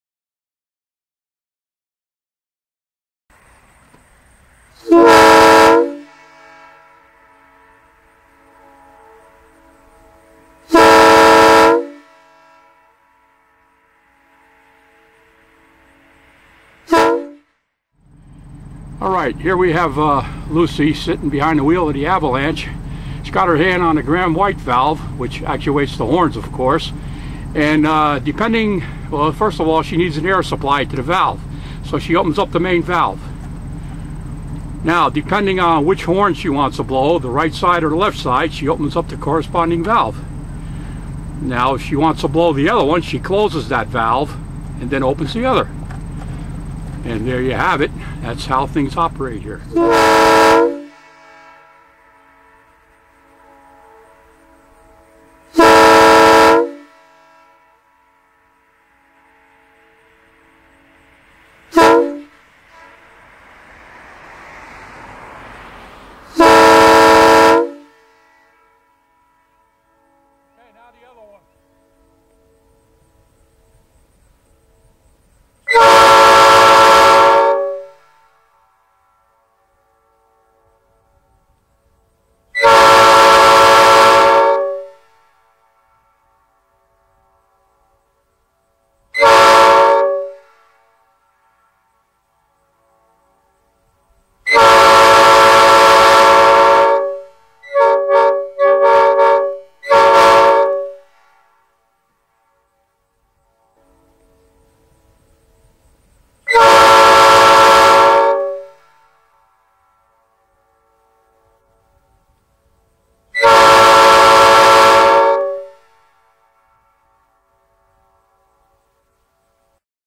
HORNS INC.
Lucy, and I are up at the Canadian Pacific former Lackawanna RR. Tunkhannock viaduct doing a photo shoot for my website. We sound off my Nathan M-3, and P-5.
Ed K. OH WELL PRODUCTIONS